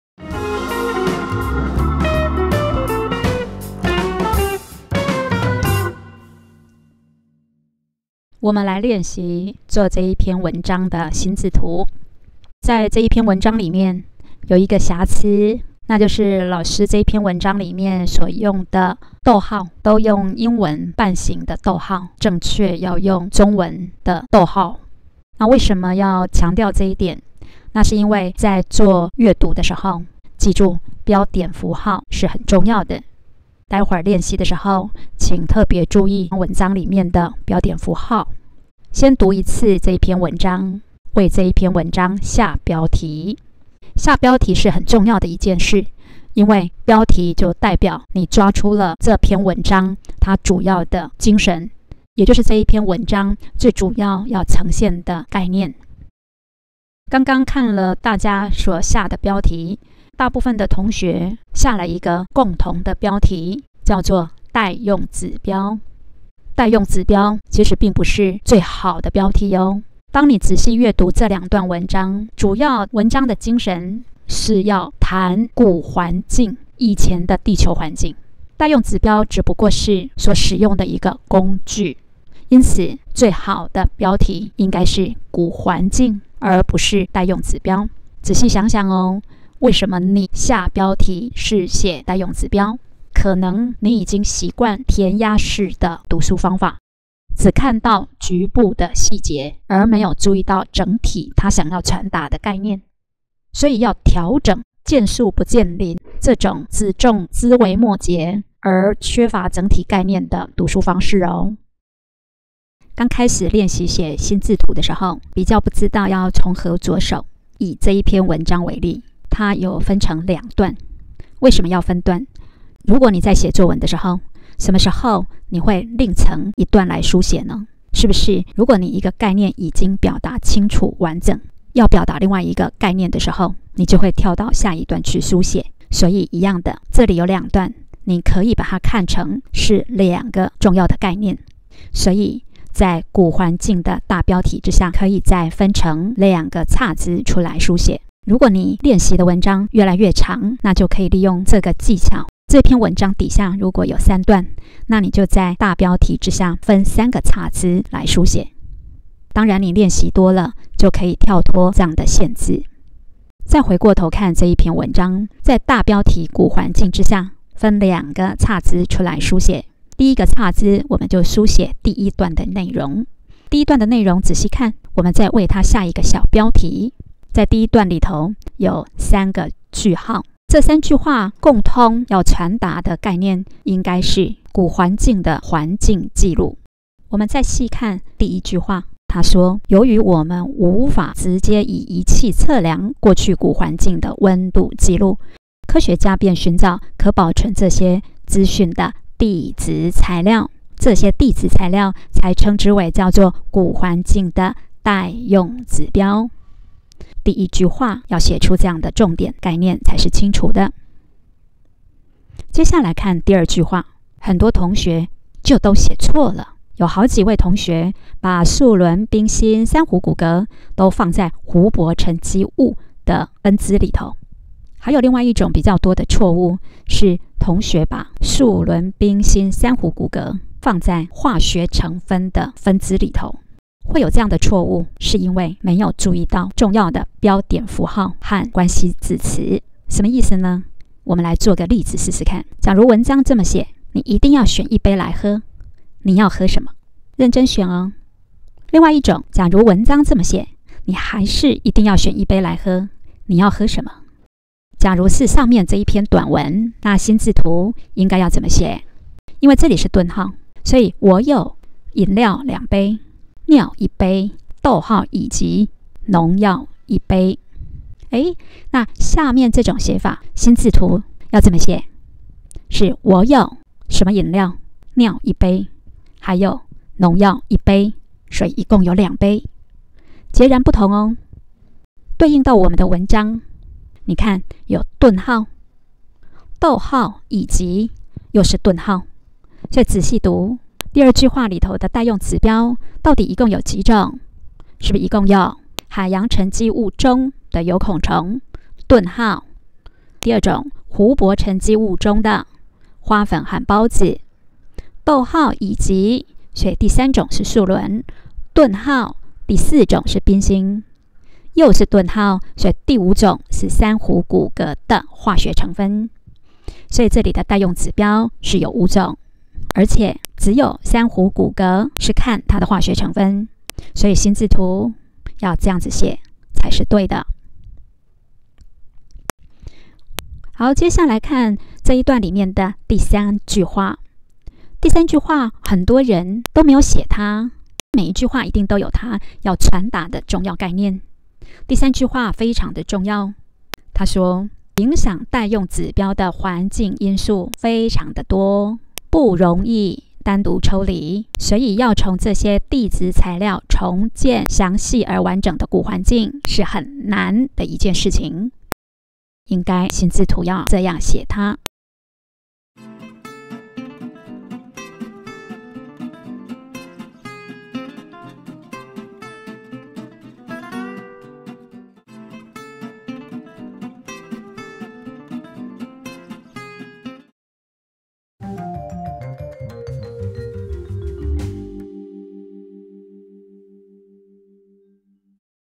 閱讀策略與心智圖- 實作技巧part5
聖功女中 吳美琍老師
閱讀策略與心智圖的實作練習與技巧
講課內容：說明閱讀時如何善用標點符號、關係字詞，以及心智圖書寫時如何訂標題等技巧！